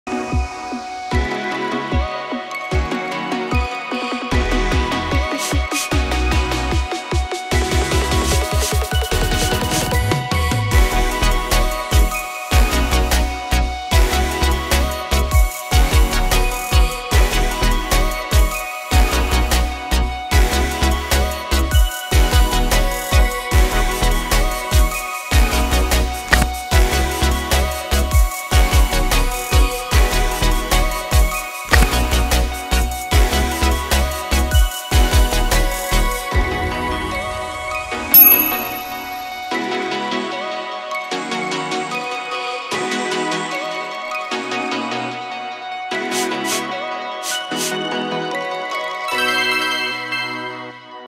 ASMR Makeup Animation | Easy Makeup Look With Red Lips 👄 | SUGAR Cosmetics
ASMR Makeup Animation - Makeup & animation has been that super fun combo that we love trying out for you lovelies. So here's one easy makeup look where you get to learn how to pull off that red lips in style.